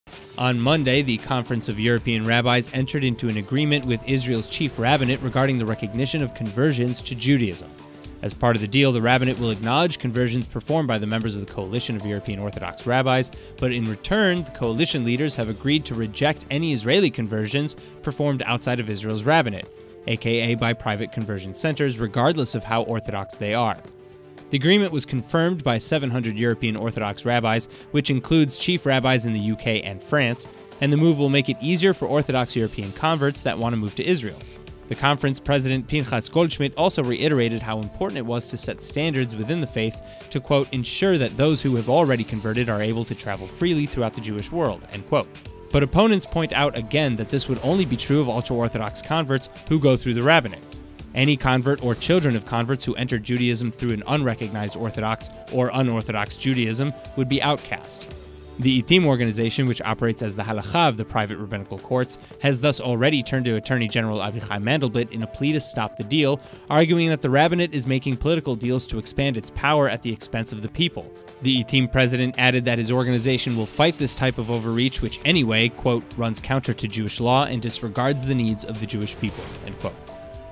European Rabbis enter into deal with Rabbinate
Only conversions through state-run religious authorities will be recognized.